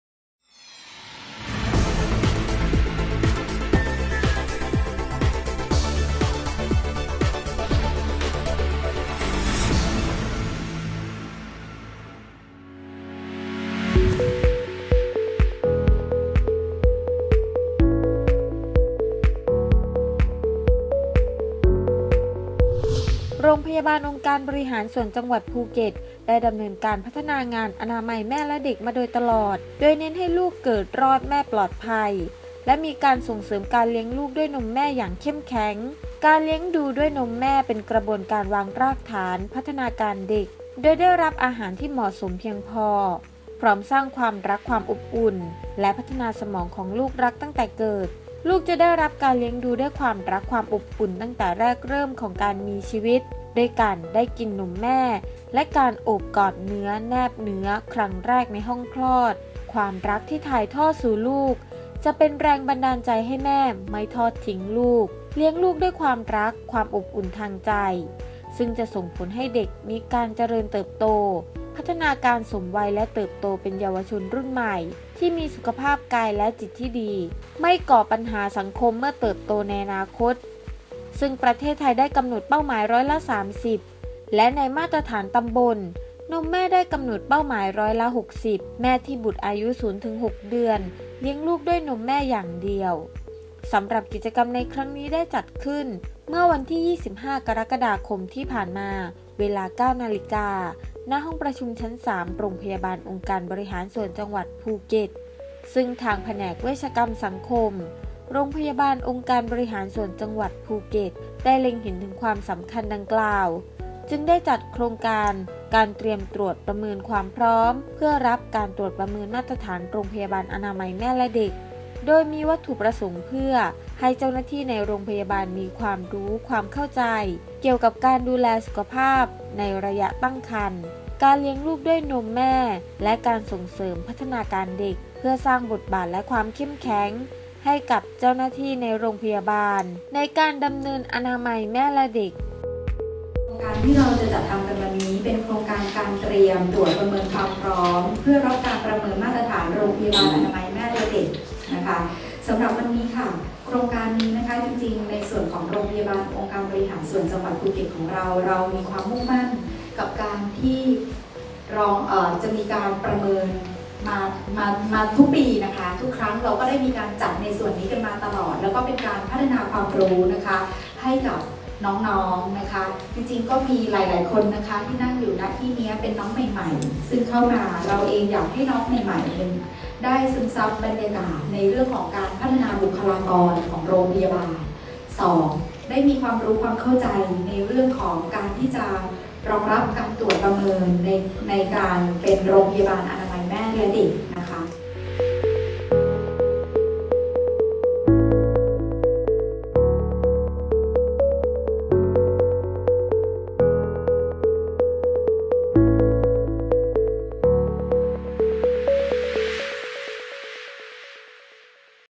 รอบรั้วอบจ.ภูเก็ต | ตรวจประเมินมาตรฐานโรงพยาบาลอนามัยแม่และเด็ก EP.25/4
โรงพยาบาลองค์การบริหารส่วนจังหวัดภูเก็ต ได้ดำเนินงานพัฒนางานอนามัยแม่และเด็กมาโดยตลอด โดยเน้นให้ลูกเกิดรอดแม่ปลอดภัยและมีการส่งเสริมการเลี้ยงลูกด้วยนมแม่อย่างเข้มแข็ง การเลี้ยงดูด้วยนมแม่เป็นกระบวนการวางรากฐานพัฒนาการเด็ก โดยได้รับอาการที่เหมาะสมเพียงพอพร้อมสร้างความรักความอบอุ่น และพัฒนาสมองของลูกตั้งแต่แรกเกิด ลูกจะได้รับการเลี้ยงดูด้วยความรักความอบอุ่นตั้งแต่แรกเริ่มของการมีชีวิตด้วยการได้กินนมแม่และการโอบกอดเนื้อแนบเนื้อครั้งแรกในห้องคลอด ความรักที่ถ่ายทอดสู้ลูกจะเป็นแรงบันดาลใจให้แม่ไม่ทอดทิ้งลูก เลี้ยงลูกด้วยความรักความอบอุ่นทางใจ ซึ่งจะส่งผลให้เด็กมีการเจริญเติบโต พัฒนาการสมวัยและเติบโตเป็นเยาวชนรุ่นใหม่ ที่มีสุขภาพกายและจิตที่ดีไม่ก่อปัญหาสังคมเมื่อเติบโตในอนาคต  ซึ่งประเทศไทยได้กำหนดเป้าหมาย ร้อยละ 30 และในมาตรฐานตำบลนมแม่ได้กำหนดเป้าหมาย ร้อยละ 60 แม่ที่บุตรอายุ 0-6 เดือน เลี้ยงลูกด้วยนมแม่อย่างเดียว
 สำหรับกิจกรรมในครั้งนี้ได้จัดขึ้นเมื่อวันที่ 25 กรกฎาคม 2560 เวลา 09.00 น. ณ ห้องประชุมชั้น 3 โรงพยาบาลองค์การบริหารส่วนจังหวัดภูเก็ตซึ่งทางแผนกเวชกรรมสังคม โรงพยาบาลองค์การบริหารส่วนจังหวัดภูเก็ต ได้เล็งเห็นถึงความสำคัญดังกล่าว จึงได้จัดโครงการ การเตรียมตรวจประเมินความพร้อม เพื่อรับการตรวจประเมินมาตรฐานโรงพยาบาลอนามัยแม่และเด็ก โดยมีวัตถุประสงค์เพื่อให้เจ้าหน้าที่ในโรงพยาบาลมีความรู้ความเข้าใจ เกี่ยวกับการดูแลสุขภาพในระยะตั้งครรภ์ การเลี้ยงลูกด้วยนมแม่และการส่งเสริมพัฒนาการเด็ก เพื่อสร้างบทบาทและความเข็มแข็งให้กับเจ้าหน้าที่ในโรงพยาบาลในการดำเนินอนามัยแม่และเด็ก